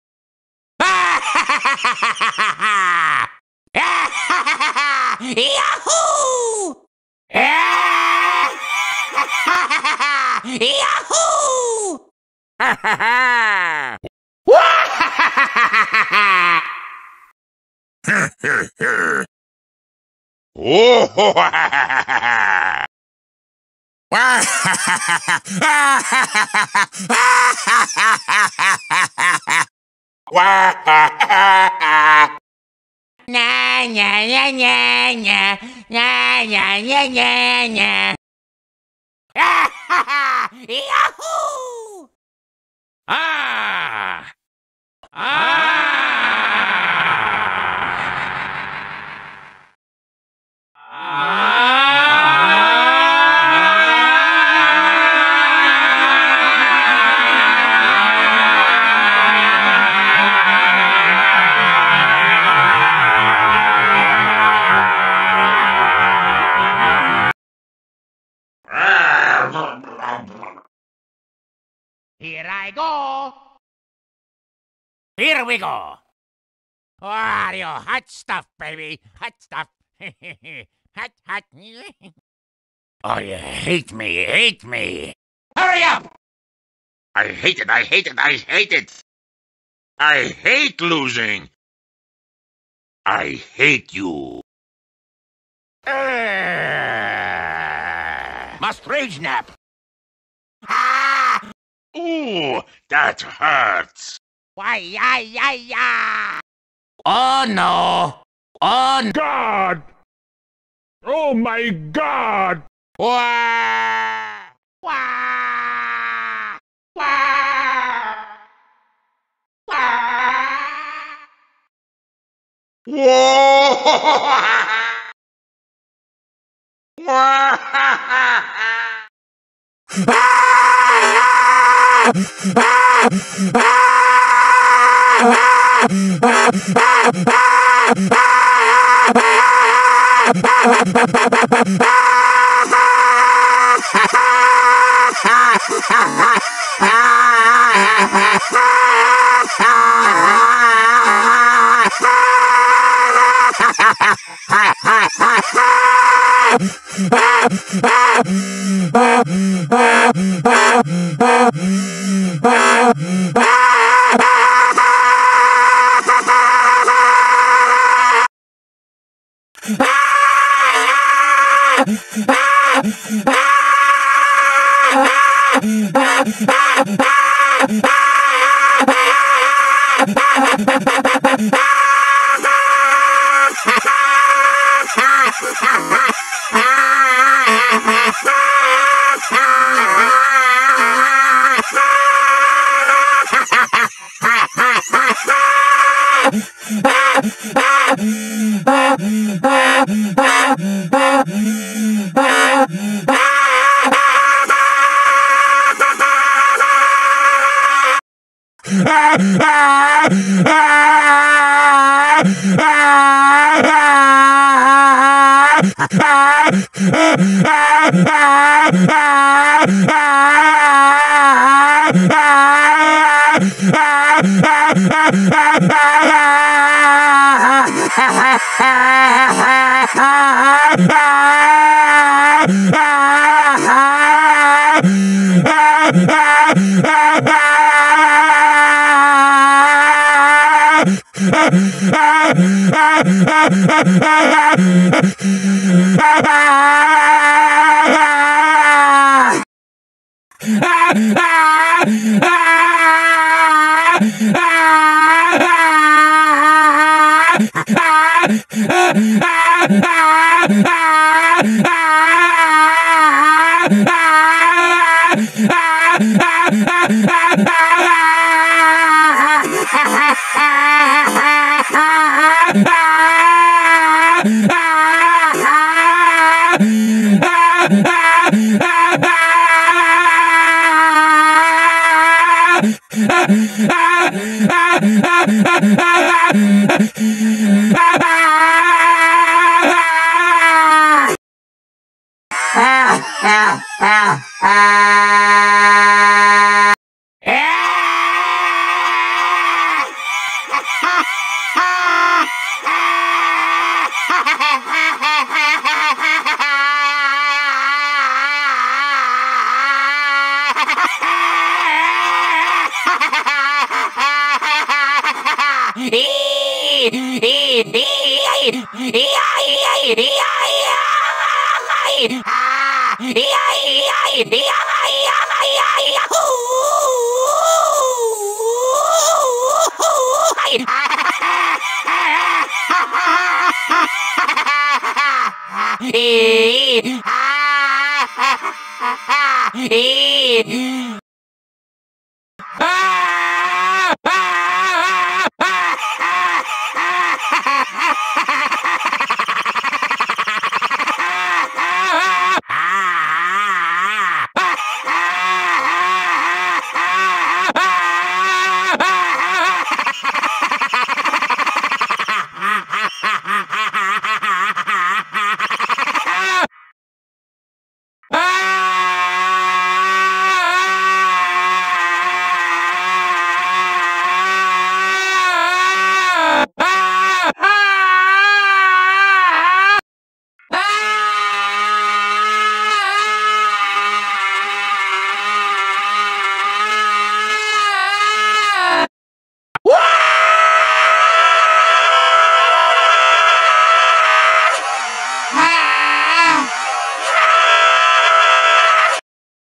wario sounds voices clips and screams custom
Wario las voces rie cansados reclamar y gritos

wario voices edited me voices clip by Manny the Headless Meme-Horse